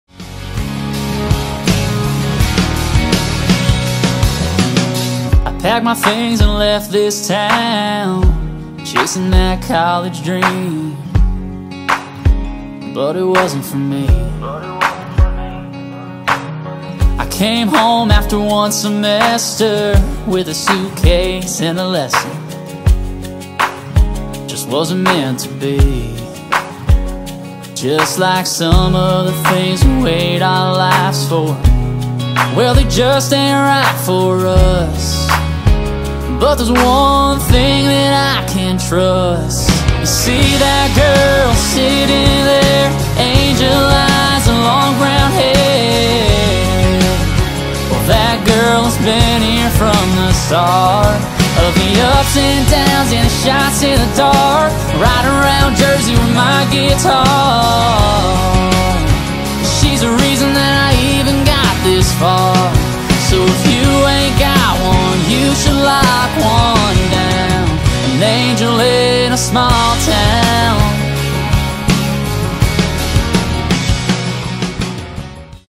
Gary Frost & Band Promo Video 2021
Gary Frost - Country Music Singer / Songwriter from Colts Neck NJ

Shots taken from a live show at The Crossroads in Garwood New Jersey